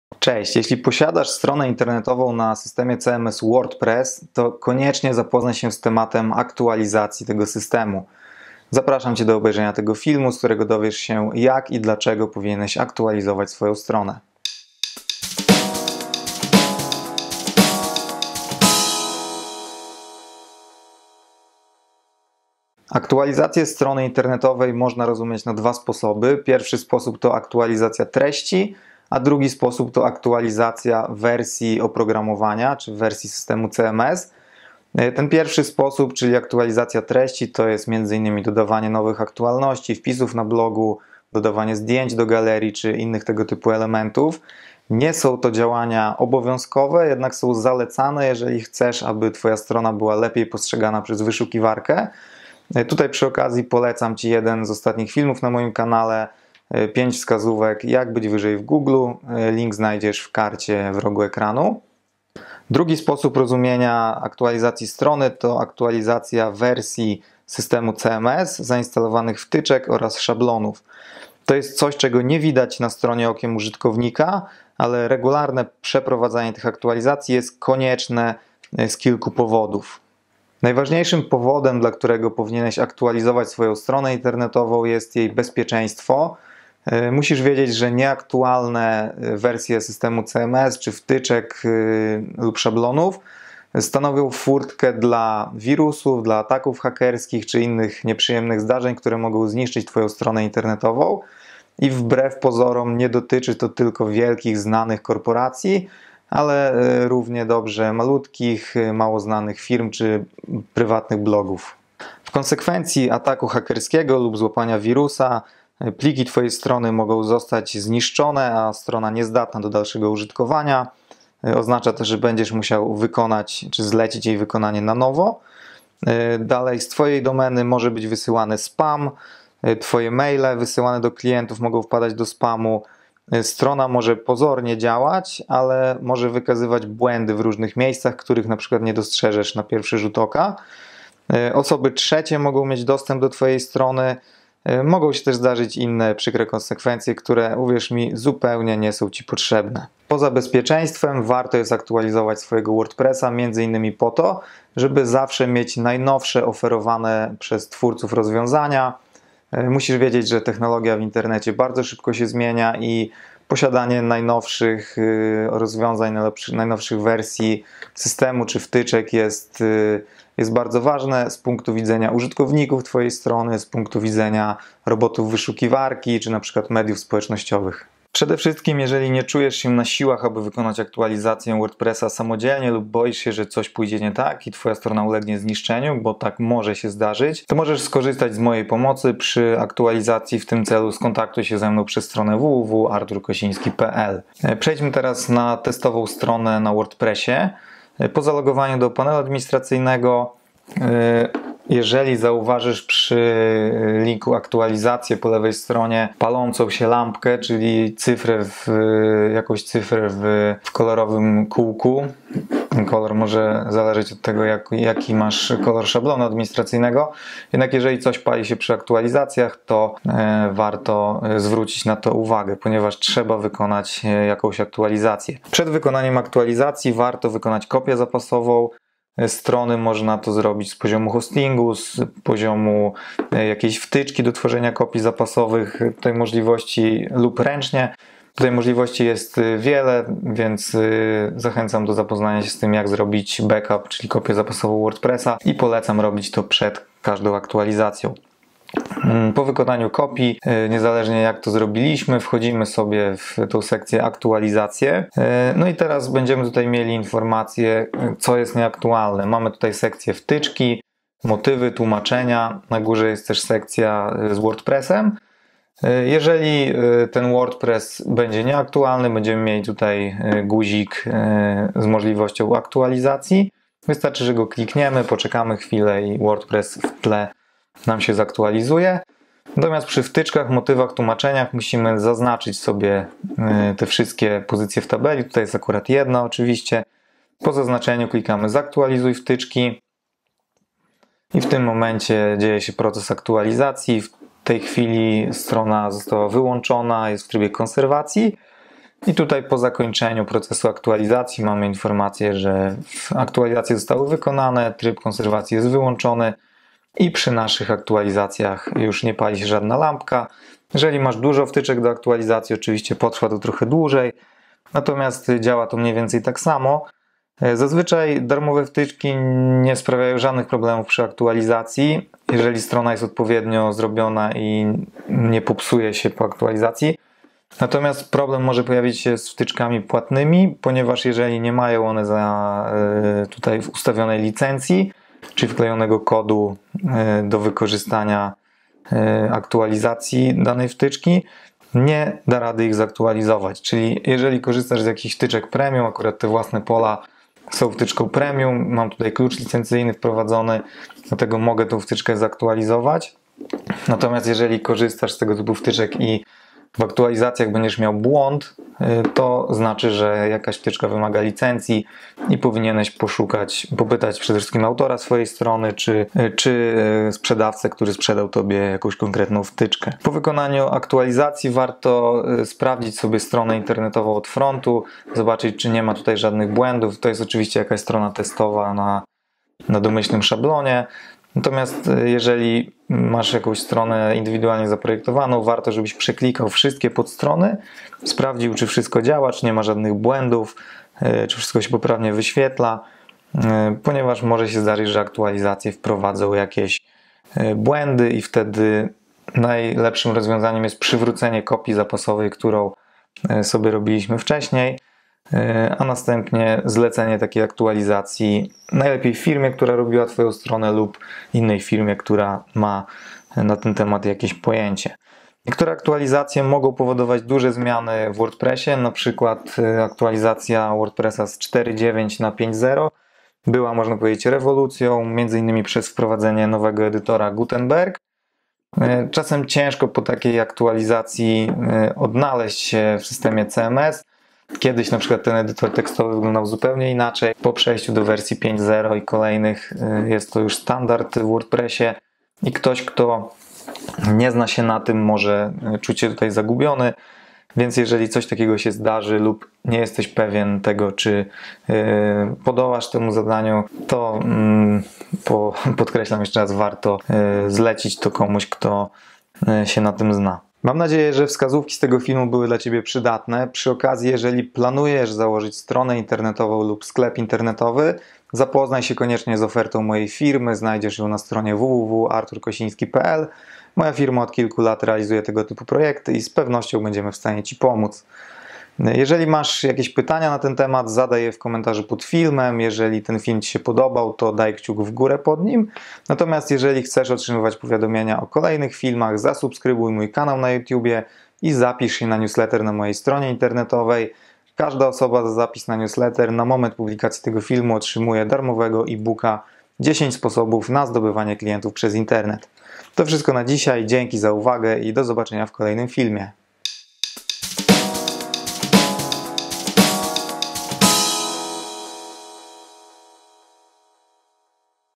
👨‍💻 Aktualizacje Wordpressa - jak i dlaczego powinieneś je robić
Cześć, jeśli posiadasz lub planujesz założyć stronę na Wordpressie to koniecznie zapoznaj się z tematem aktualizacji tego systemu. W tym filmie powiem Ci jak i dlaczego powinieneś aktualizować swoją stronę. Zapraszam do oglądania.

Co to jest aktualizacja WordPress?

Aktualizację strony można rozumieć na dwa sposoby.

Pierwszy sposób to aktualizacja treści, czyli dodawanie nowych np. wpisów na blogu, aktualności, zdjęć do galerii, itd. To działanie nie jest konieczne, ale jest wskazane jeśli chcesz, aby Twoja strona była lepiej postrzegana przez Google. Przy okazji możesz zobaczyć inny mój film „5 wskazówek jak być wyżej w google”.

Drugi sposób rozumienia to  aktualizacja wersji systemu CMS, zainstalowanych wtyczek oraz szablonów. To coś, czego nie widać na stronie okiem użytkownika, ale regularne przeprowadzanie tych aktualizacji jest konieczne z kilku powodów.

Dlaczego trzeba aktualizować WordPressa?

Najważniejszym powodem, dla którego powinieneś aktualizować swojego WordPressa to bezpieczeństwo strony. Nieaktualne wtyczki, szablony czy sam system stanowią zagrożenie ataku hakerskiego czy złapania wirusa. I nie dotyczy to tylko stron wielkich korporacji, ale nawet malutkich nieznanych blogów.

W konsekwencji ataku hakerskiego lub wirusa:

- pliki Twojej strony mogą zostać zniszczone a strona niezdatna do dalszego użytkowania,
- z Twojej domeny może być wysyłany spam,
- Twoje maile wysyłane do klientów mogą wpadać do spamu,
- strona może pozornie działać, ale wykazywać błędy w różnych miejscach,
- osoby trzecie mogą mieć dostęp do Twojej strony,
- inne przykre konsekwencje, które zupełnie nie są Ci potrzebne.